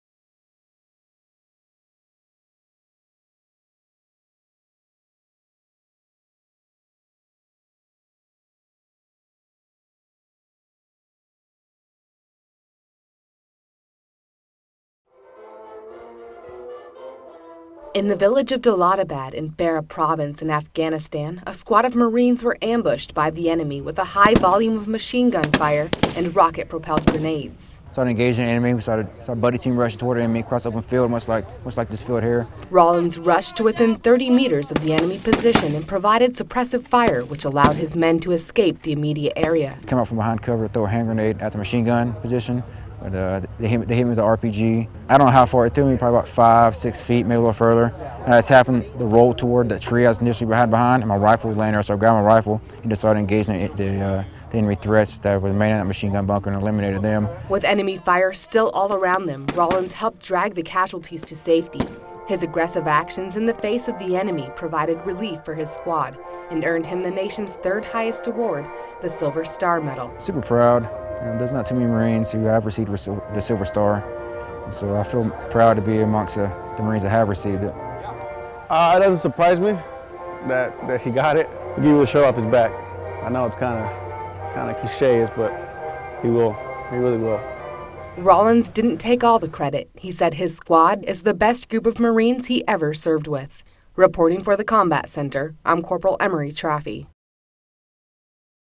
USMC Silver Star | Sgt William Rollins
Package about the heroic actions of Sgt. William Rollins. His valor in Afghanistan earned him the Silver Star. The Nation's 3rd highest award for gallantry in combat was presented to him during a ceremony at the Marine Corps Air Ground Combat Center, Twentynine Palms, California, Dec. 17, 2010.

Across the branches, the Silver Star is the third highest military decoration that can be awarded for valor in the face of the enemy.

Sgt. Rollins of Houston, Texas, was on reconnaissance patrol in Afghanistan, in June of 2008, when his Marine squad was ambushed by Taliban fighters using machine guns and rocket-propelled grenades.

Sgt. Rollins, the squad leader, engaged the enemy "in the face of certain death" and provided suppressive gunfire that allowed his Marines to escape the ambush. Moving through enemy fire, Rollins helped pull a wounded Marine to safety.

Assigned to the 2nd Battalion, 7th Marine Regiment (2/7) from Twentynine Palms, Rollins is now out of the military.

The Silver Star Awards ceremony was held Dec. 17 at the Marine base and attended by his wife, parents and 200 Marines.

Interviews:
Sgt. William Rollings, Silver Star Recipient
Edgar Figueroa, Rollings' Friend